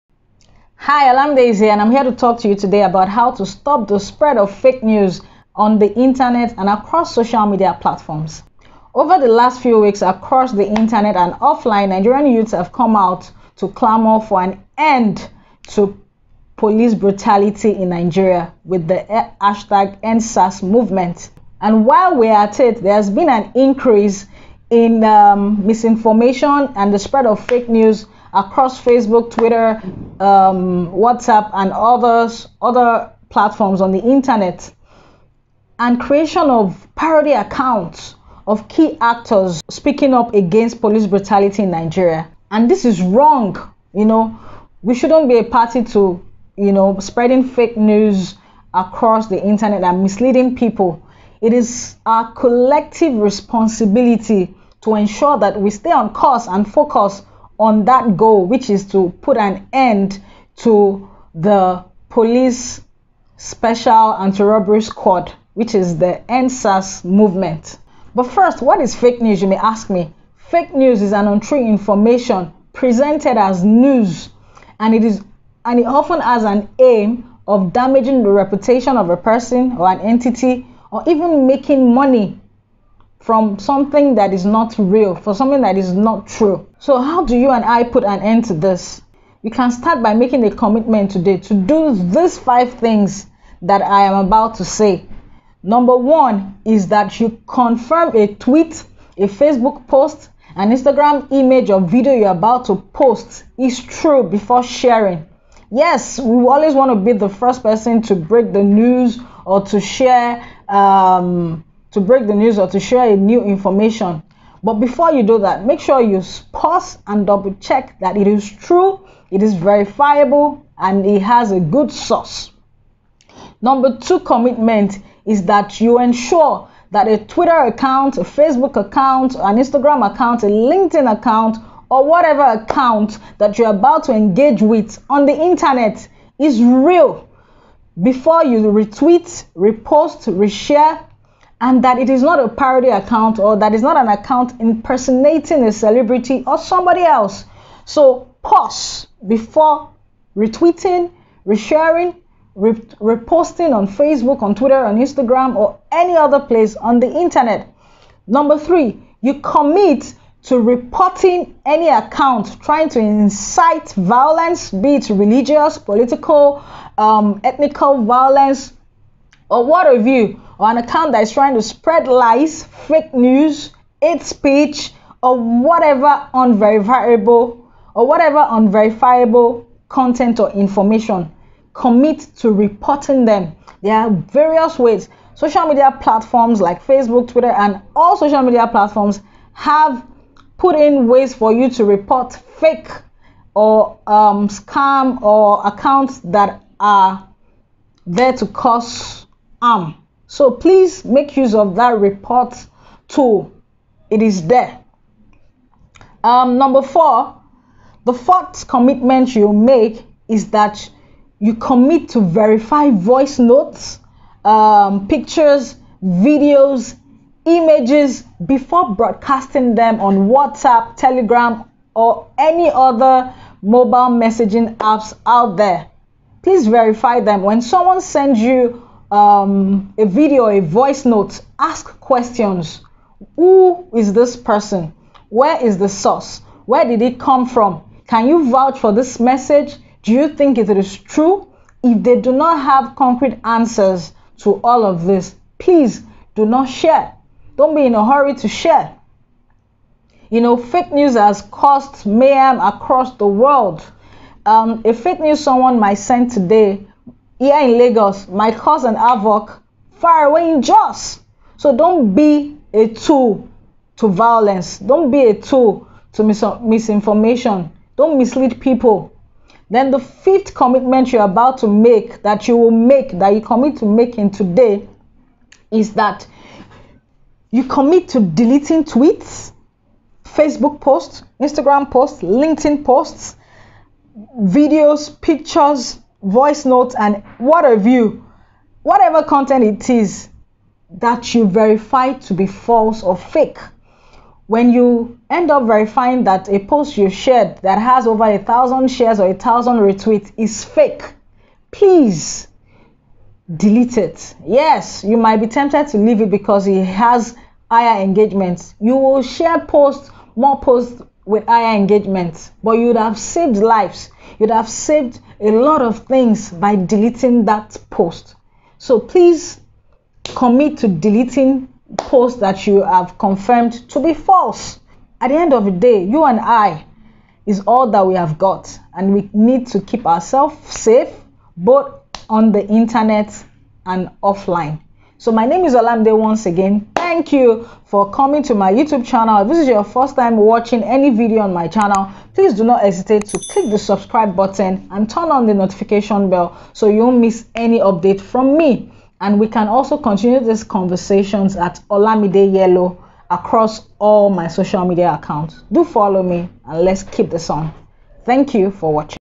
How To Stop The Spread Of Fake News On Social Media
In light of the current call to EndSARS in Nigeria, this video shows how to STOP FAKE NEWS spread across social media. There has been widespread of misinformation and fake news over the last 2 weeks, here is how we can collectively stop the SPREAD